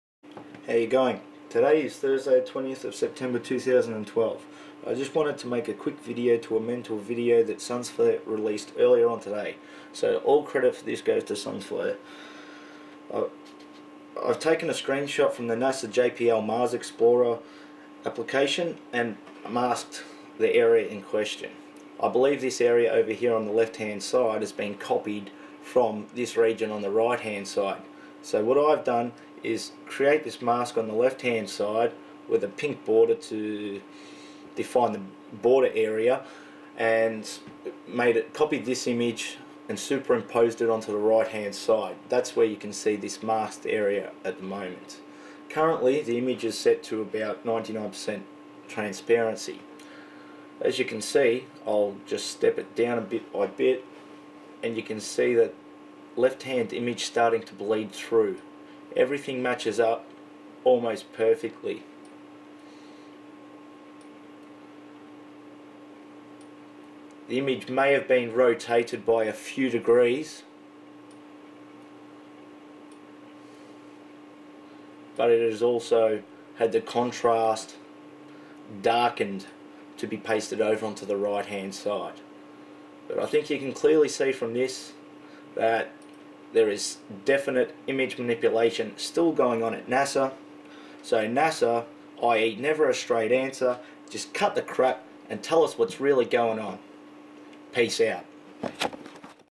NASA JPL Mars Curiosity Image Manipultion
Never A Straight Answer caught out manipulating images once again

It can be found near the Landing Area

NASA Mars Reconnaissance Orbiter HiRISE image (July 9th 2008)

NASA Mars Reconnaissance Orbiter HiRISE image (July 20th 2008)

Google Earth Mars Coordinates: 4°44'50.96"S 137°22'41.37"E

- Earthfiles Reported by Linda Moulton Howe

"2MIN News Sept 28, 2012: NASA Fakes Mars Photos!" Suspicious0bservers